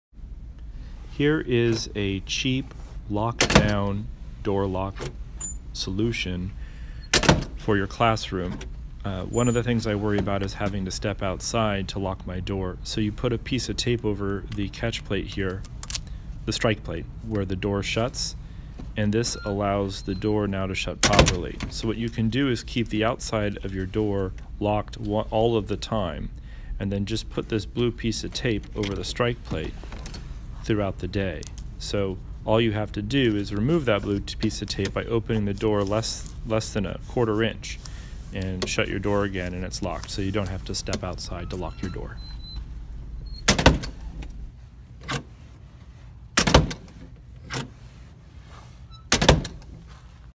Lockdown Classroom Door lock
One of the things I worry about is having to lock my door from the outside during an emergency. This is a nearly free solution to that problem. This is useful for any teacher who is in a similar situation where his or her door cannot be locked from the inside